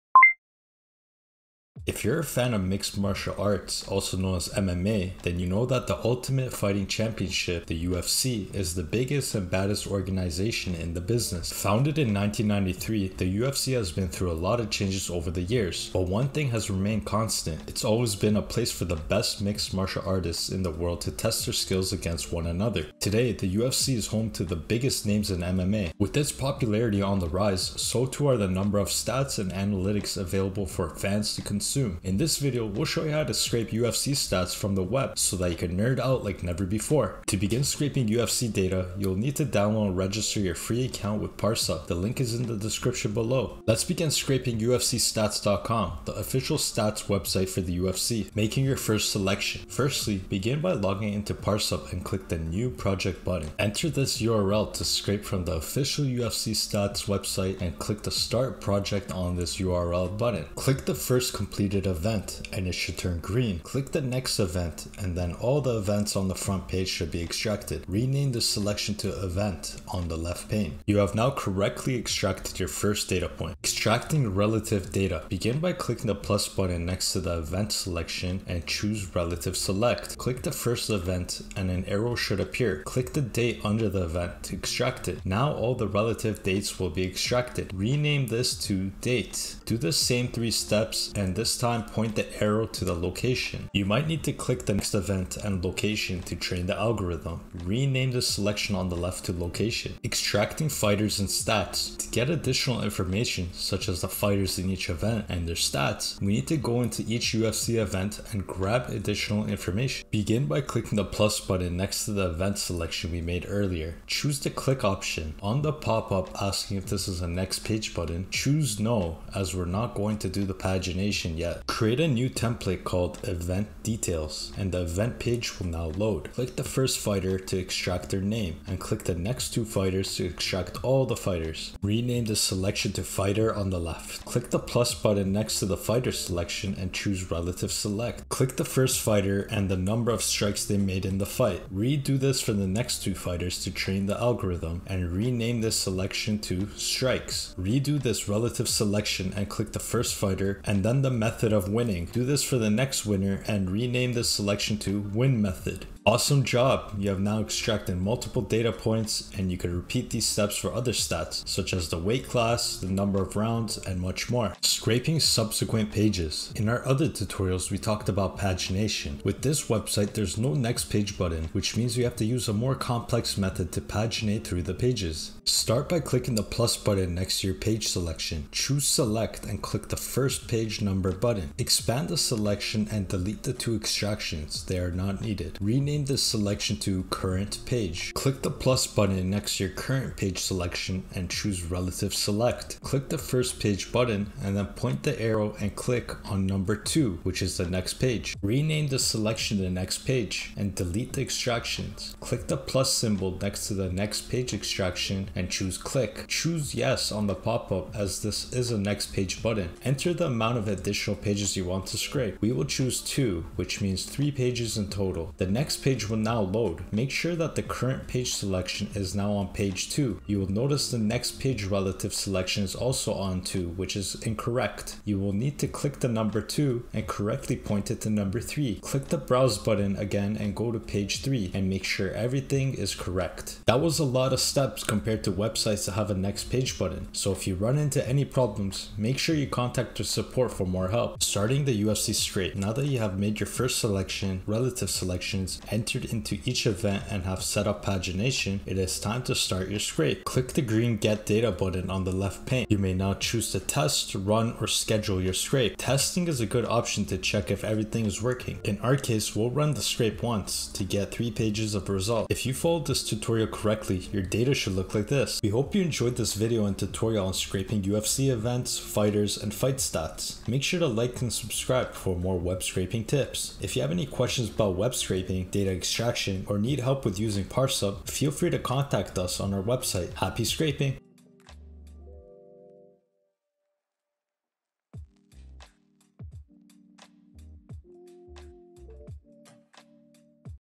How to Scrape UFC Stats
Today, the UFC is home to some of the biggest names in MMA. With its popularity on the rise, so too are the number of stats and analytics available for fans to consume. In this video, we'll show you how to scrape UFC stats from the web so that you can nerd out like never before!

0:00 Intro
0:48 Making Your First Selection
1:14 Extracting Relative Data
1:40 Extracting Fighters and Stats
2:45 Pagination
4:01 Starting The Scrape